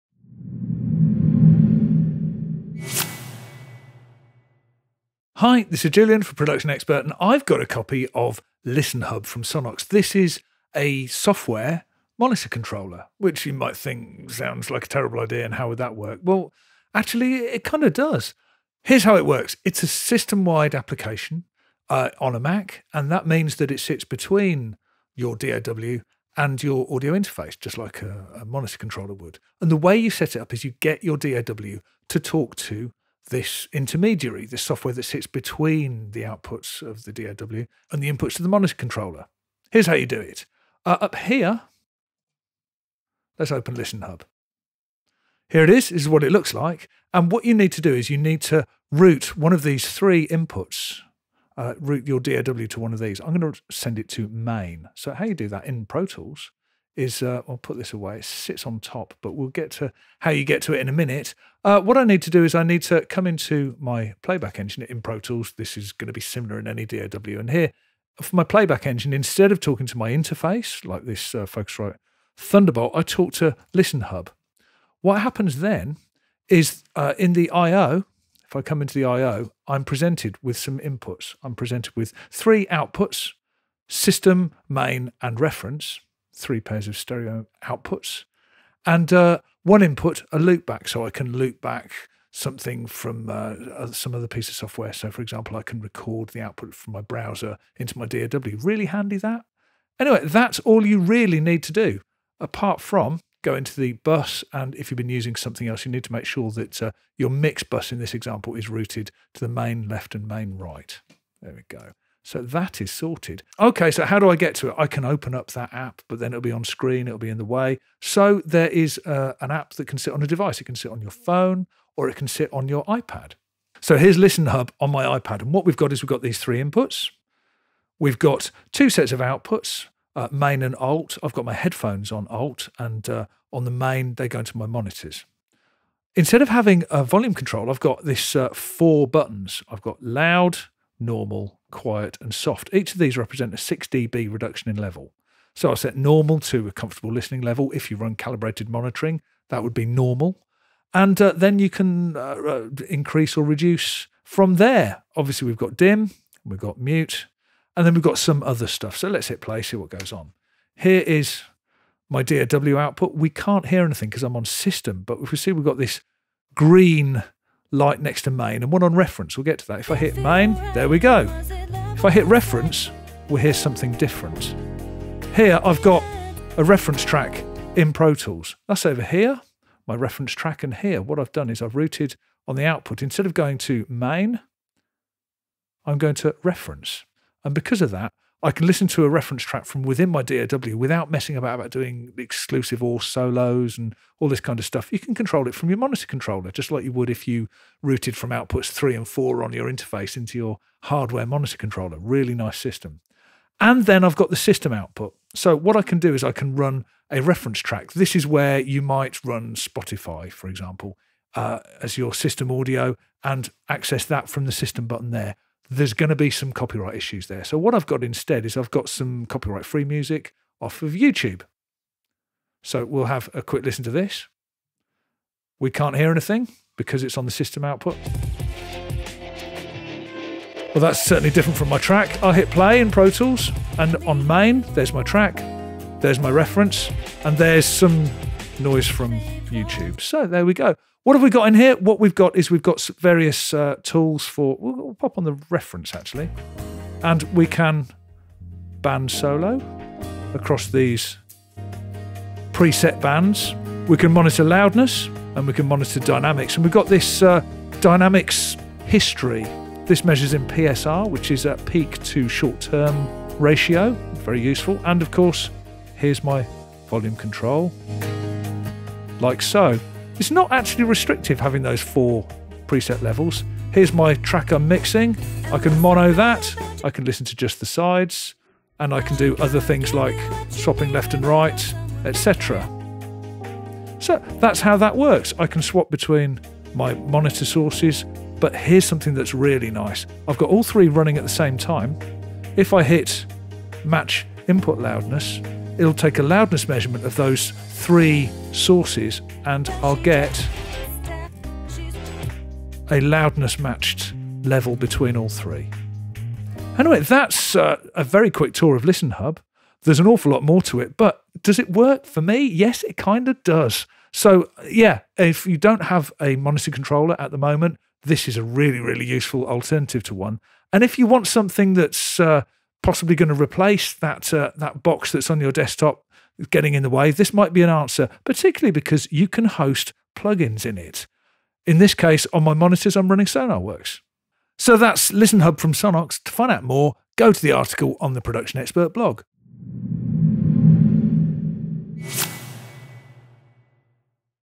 Sonnox ListenHub - Who Said Monitor Controllers Have To Be Hardware?
In this video Julian looks at ListenHub from Sonnox, a very capable alternative to a hardware monitor controller.